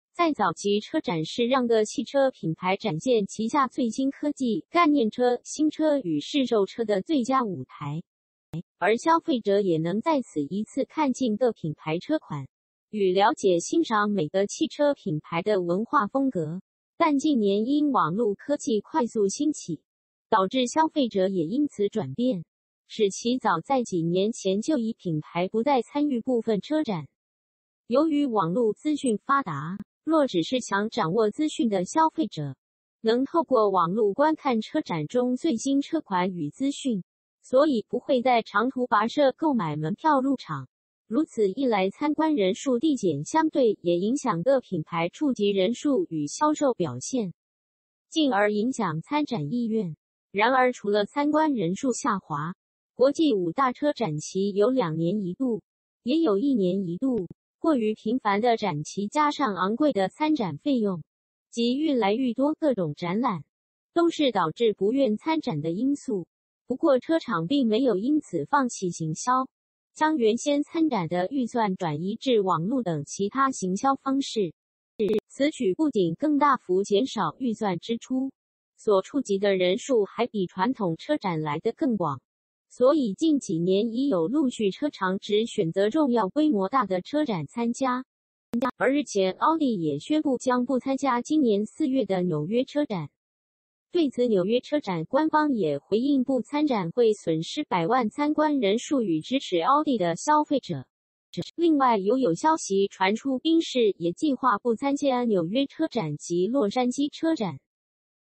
Audi不玩了，宣布退出紐約車展，其它家會跟進嗎？
在早期車展是讓各汽車品牌展現旗下最新科技、概念車、新車與市售車的最佳舞台，而消費者也能在此一次看盡各品牌車款，與了解欣賞每個汽車品牌的文化風格，但近年因網路科技快速興起，導致消費者也因此轉變，使其早在幾年前就已品牌不再參與部分車展。由於網路資訊發達，若只是想掌握資訊的消費者，能透過網路觀看車展中最新車款與資訊，所以不會再長途跋涉購買門票入場，如此一來參觀人數遞減相對也影響各品牌觸及人數與銷售表現，近而影響參展意願。然而除了參觀人數下滑，國際五大車展期有兩年一度，也有一年一度，過於頻繁的展期加上昂貴的參展費用，及愈來愈多各種展覽。都是導致不願參展的因素。不過車廠並沒有因此放棄行銷，將原先參展的預算轉移至網路等其它行銷方式，此舉不僅更大幅減少預算支出，所觸及的人數還比傳統車展來得更廣，所以近幾年已有陸續車廠只選擇重要規模大的車展參加。而日前Audi也宣布將不參加今年四月的紐約車展，對此紐約車展官方也回應不參展會損失百萬參觀人數與支持Audi的消費者，另外有有消息傳出賓士也計畫不參加紐約車展及洛杉磯車展。